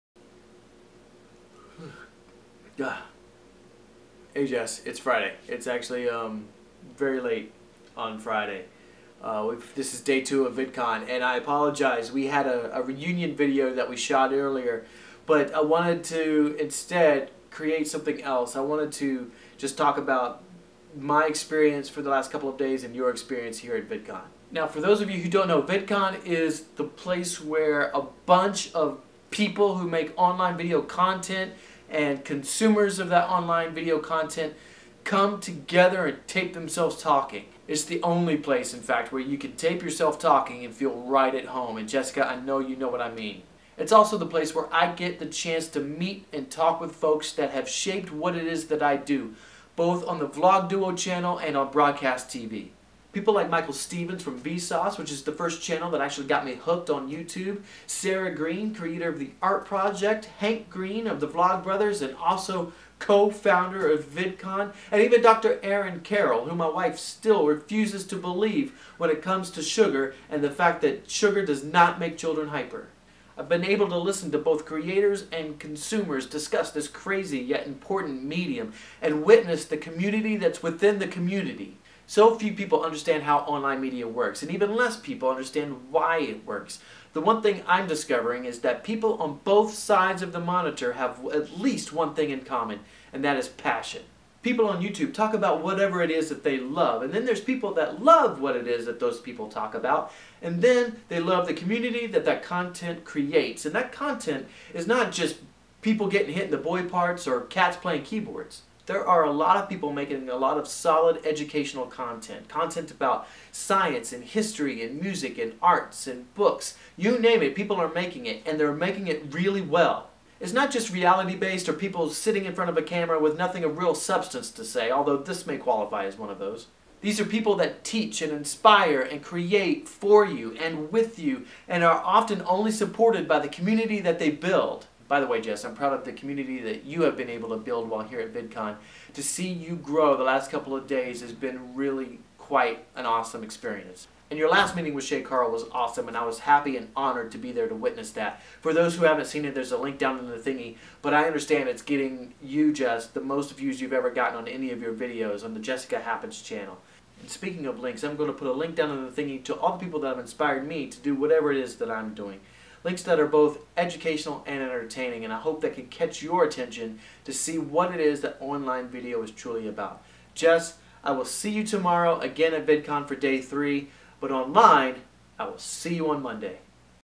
Two Days Down at VidCon 2014
Tim reflects on his first visit to VidCon and shares his thoughts on what online media truly is - or should be. (Spoiler alert: it's not just about keyboard cats.)